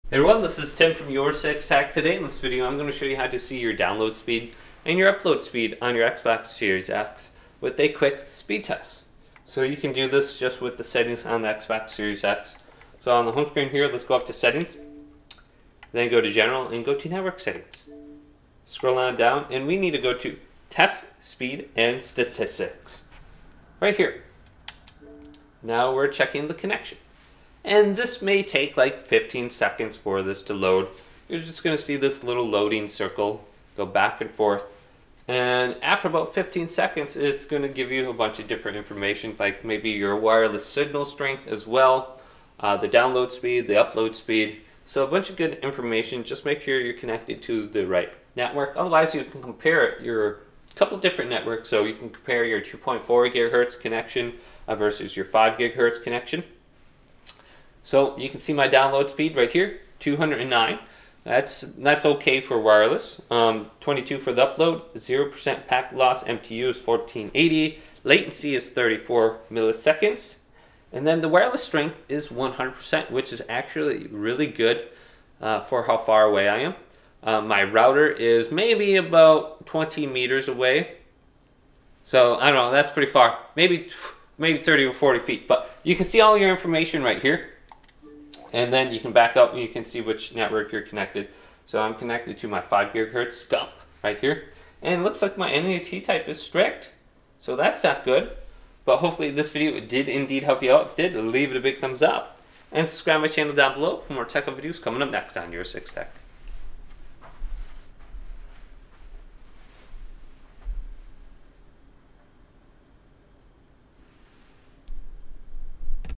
How to Perform a Speed Test for Download & Upload on Xbox Series X (Settings Tutorial)
To perform a speed test for download and upload on your Xbox Series X, follow these steps:

Navigate to Profile & system and select Settings.
Choose General and then Network settings.
Select Test network speed & statistics.
This test will assess your internet connection and display the following information:

Download speed: The rate at which data is downloaded from the internet.
Upload speed: The rate at which data is uploaded to the internet.
Latency: The delay between sending a request and receiving a response.
Keep an eye on these results to ensure optimal performance for your gaming experience! 🎮🚀

If you encounter any issues, consider checking your router settings, using a wired LAN connection, or moving your console closer to the router for better connectivity